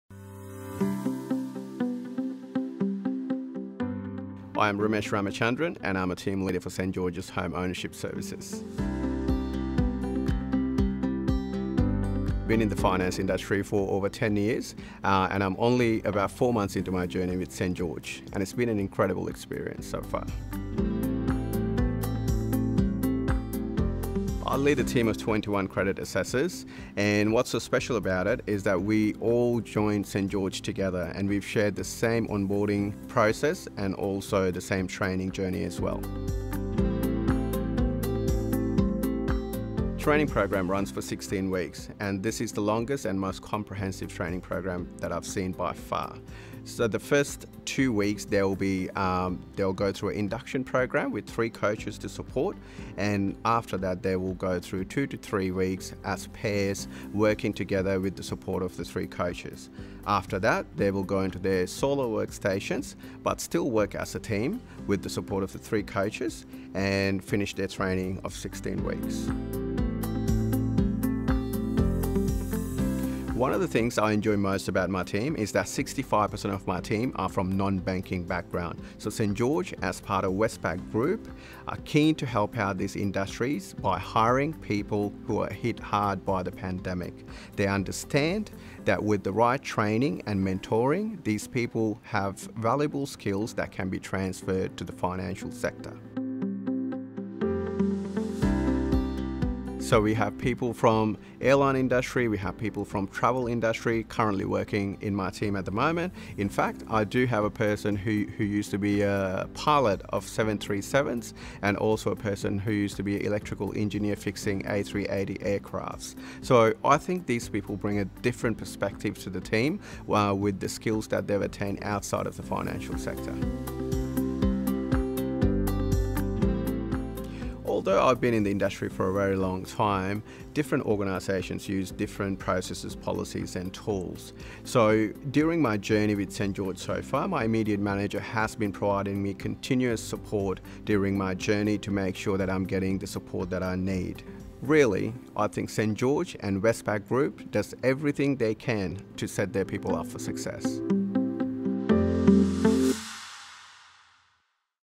Ramesh - training for success
Ramesh and his team of 20 all joined together, trained together and now stick together. Learn how our training helps you get set up for success - no matter your level of experience.